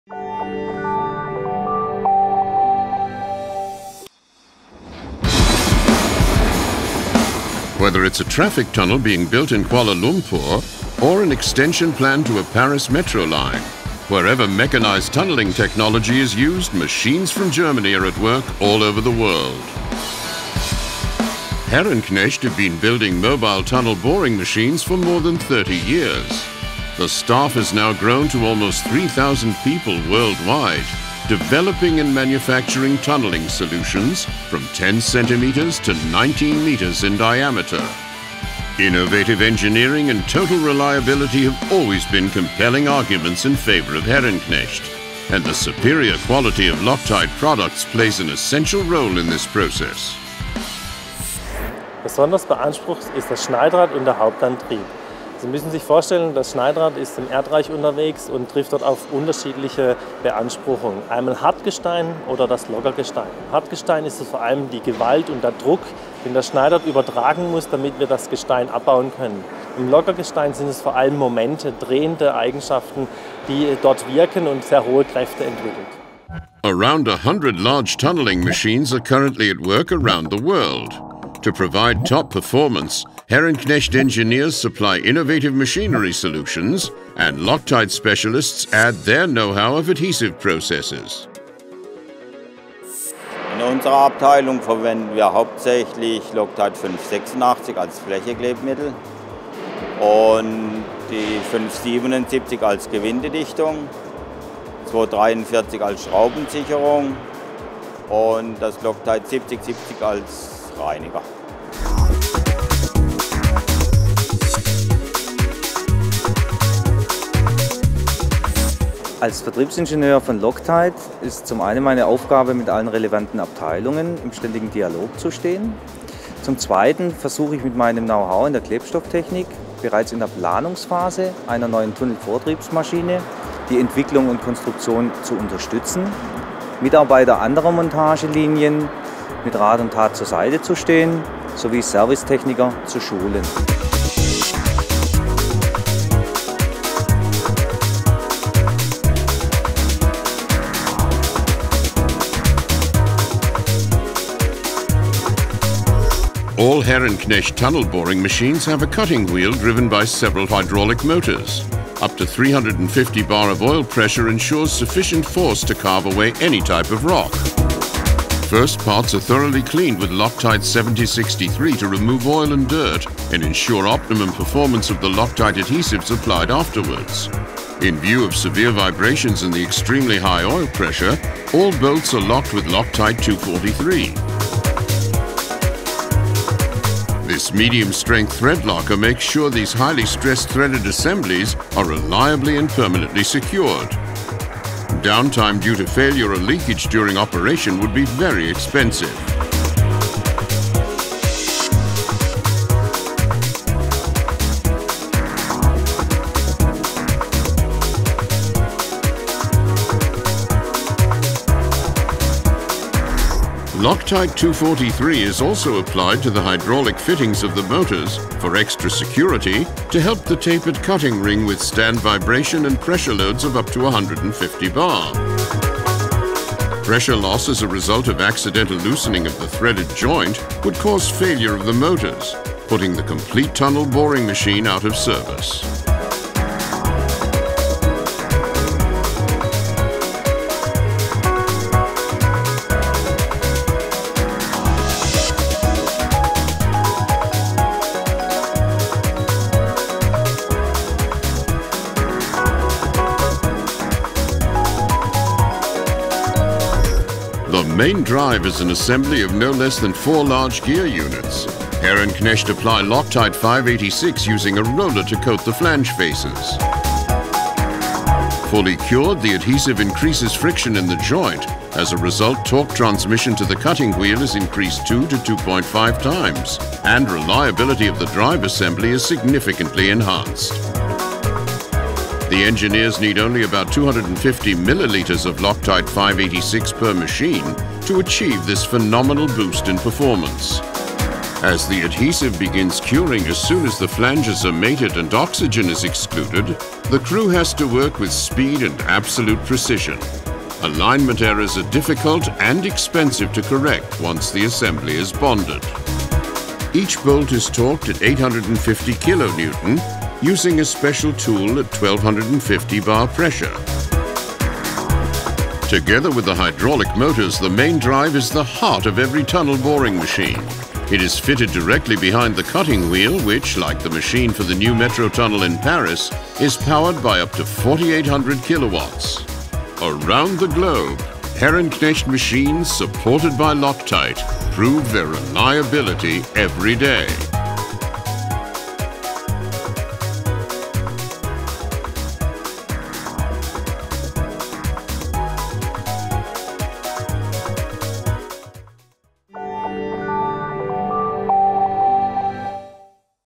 LOCTITE Success Story: Herrenknecht
Herrenknecht is the world's leading manufacturer of tunnel boring machines, with 100 machines at work on projects ranging from traffic tunnels in Kuala Lumpur to the Paris metro. Watch how LOCTITE is used.